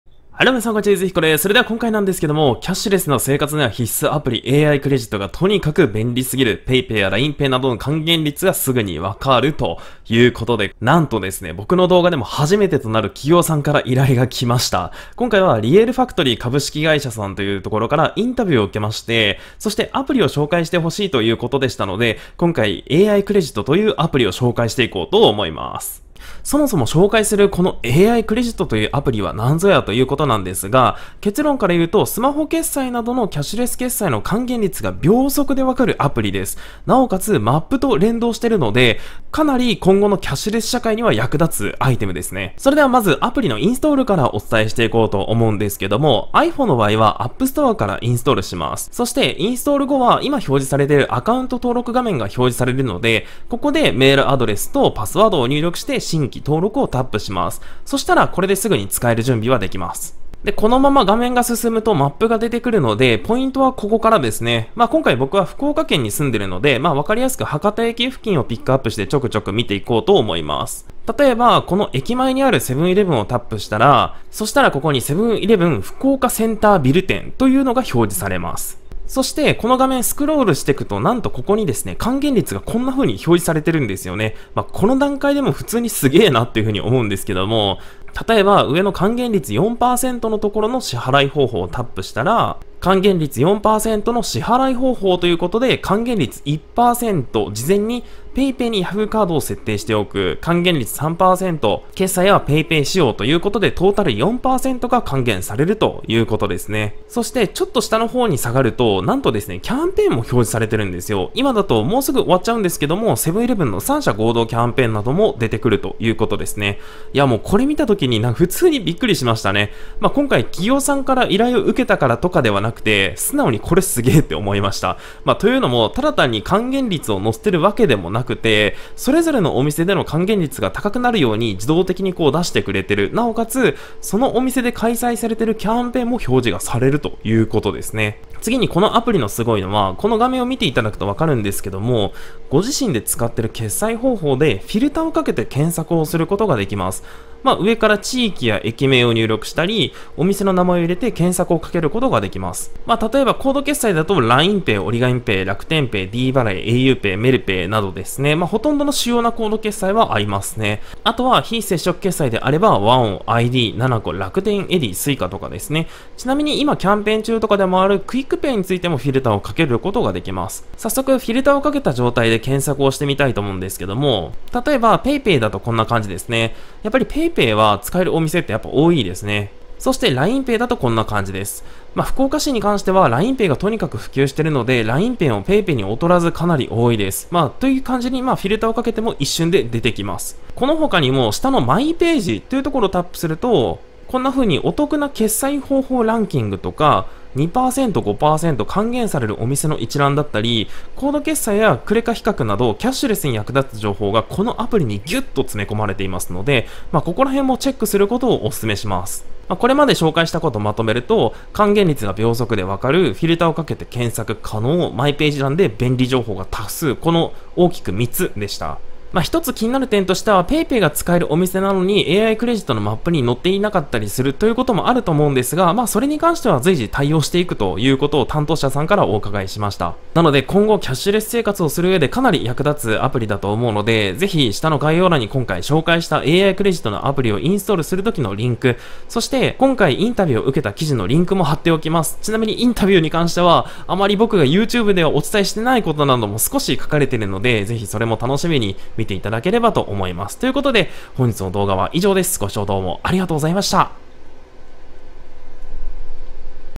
キャッシュレス(スマホ決済)な生活には必須アプリ！AI-Credutがとにかく便利すぎる！PayPayやLINEPayなどの還元率がすぐにわかる
使用機材
マイク：Rode NT1A
キャプチャーボード：I-O DATA GV-USB3
オーディオインターフェース：YAMAHA AG03

キャッシュレス スマホ決済 AI-Credut